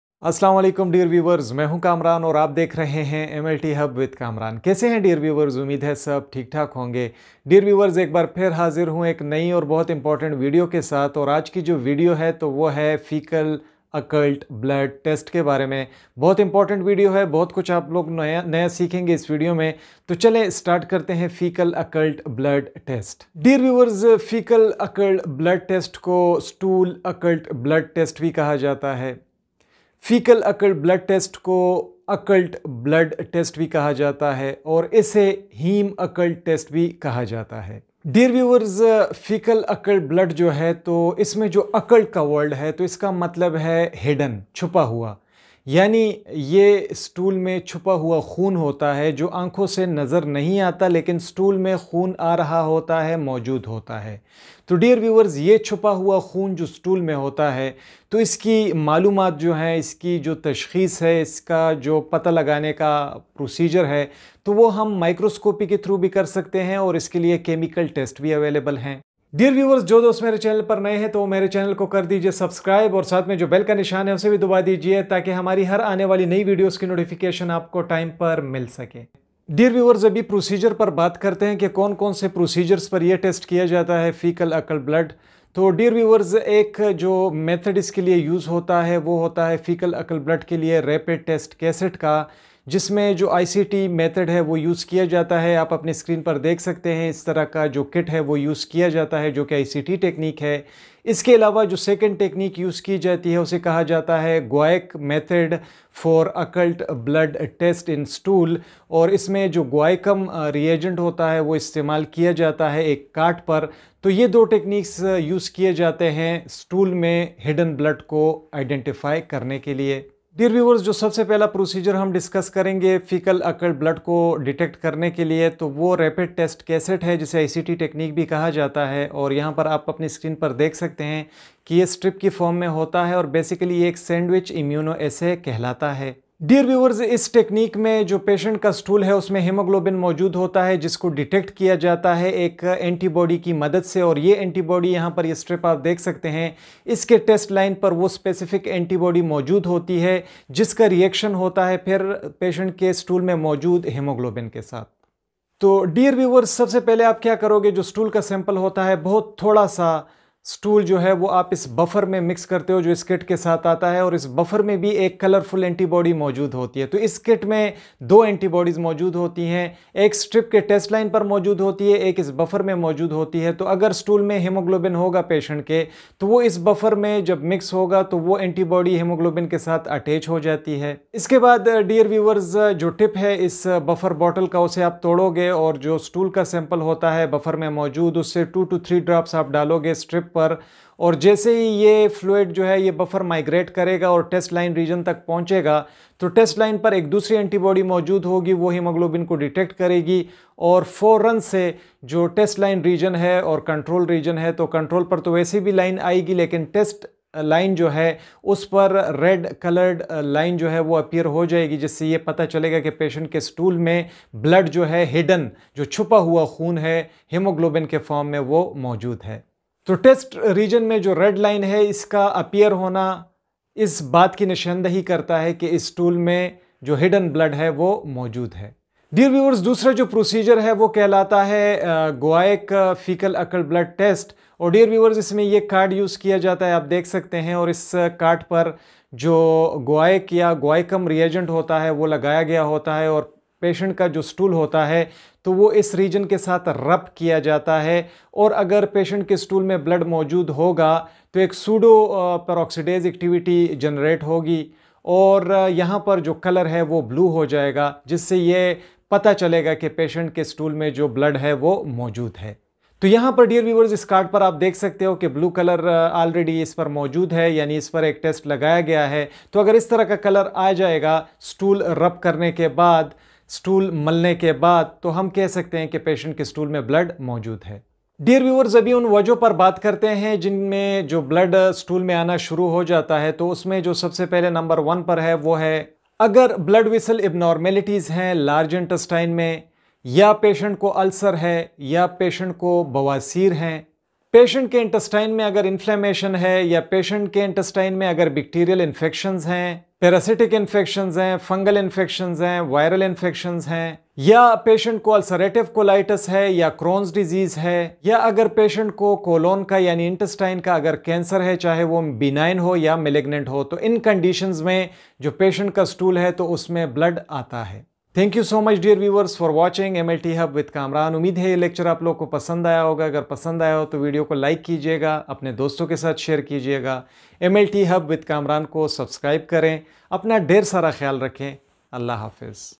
Fecal Occult Blood Test (FOB) || Stool Occult Blood Test (SOB)
Fecal occult blood is the blood in stool which is not visible by naked eye. Also called Stool Occult Blood Test (SOB).

Queries solved in this video:
00:00 | Introduction?
01:01 | Procedures for occult blood?
01:10 | Procedures for Fecal Occult Blood Rapid Cassette?
01:46 | Procedure of Guaiac faecal occult blood test?
02:16 | Causes of Blood in Stool?